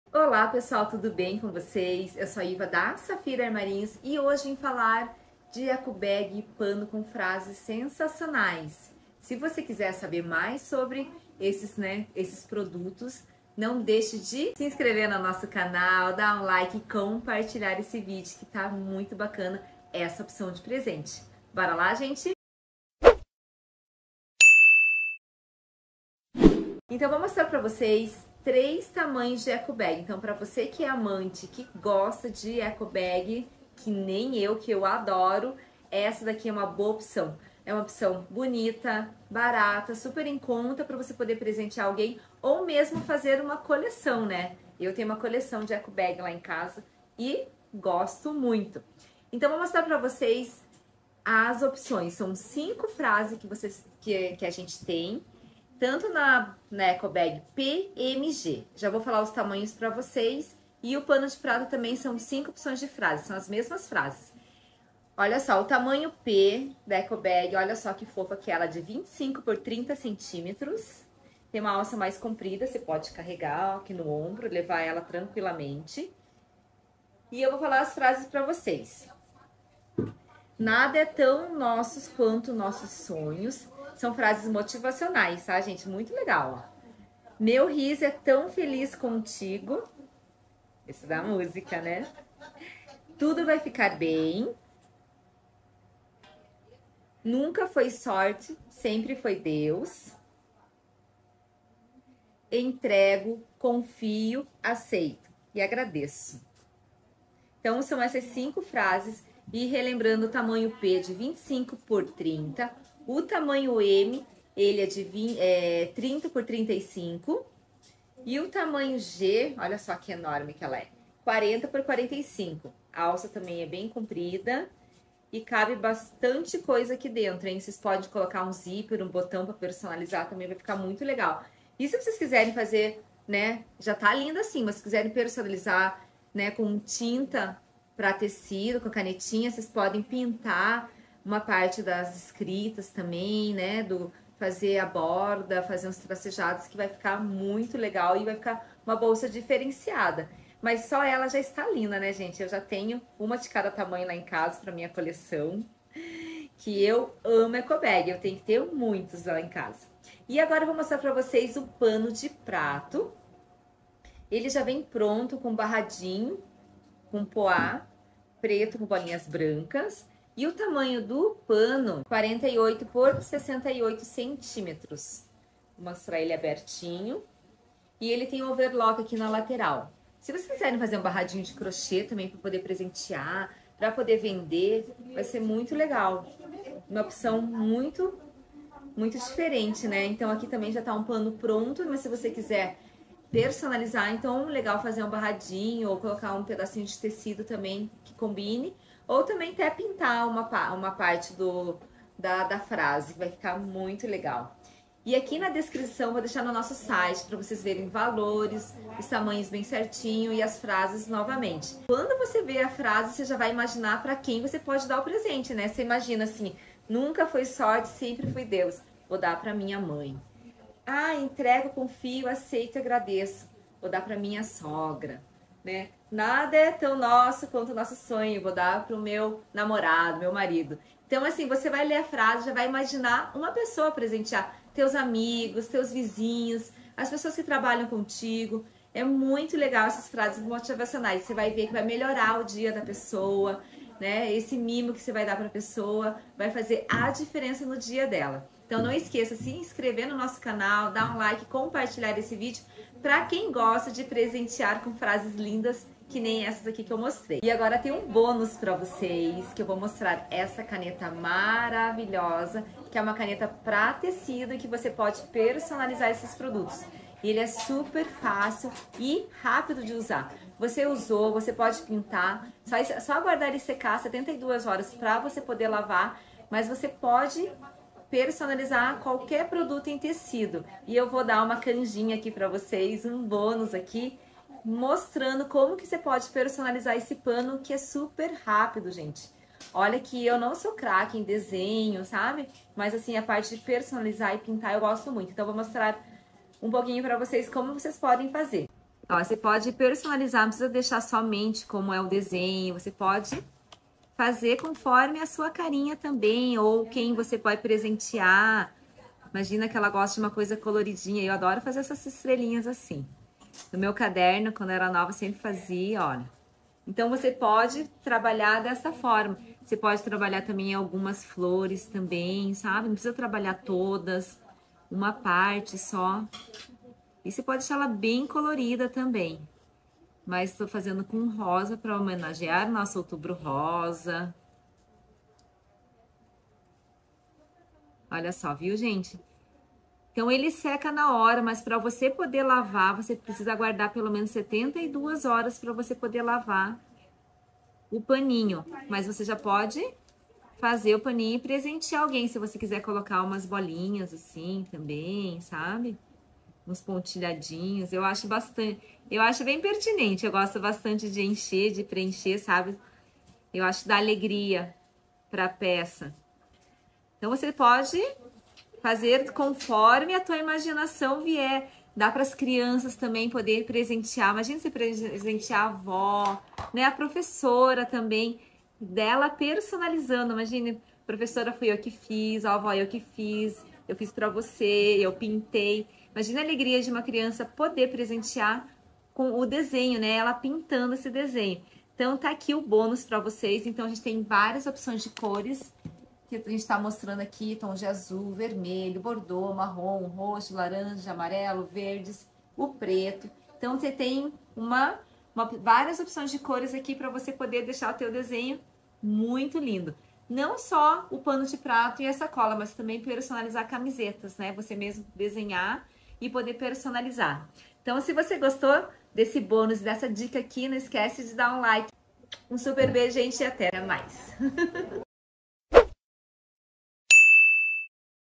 ECOBAGS E PANOS DE PRATO FRASES - PARA PRESENTEAR
Olá pessoal 🥰
Nesse vídeo trouxemos ótimas opções para presentear, inclusive nesse dia dos professores, hein? São várias opções de Ecobags e Panos de Copa com frases motivacionais, que podem melhorar o dia daquela pessoal especial 🥰 E o melhor, você pode personaliza-los e deixar com a carinha da pessoa😍

Corre já em nosso site garantir o seu hein?  💙 Enviamos para todo o Brasil 🥰
💎 Panos de Copa: Disponíveis em nosso WhatsApp Delivery (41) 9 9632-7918

🔴 Qualquer dúvida, é só entrar em contato conosco, ok? Estamos à disposição! 😉💙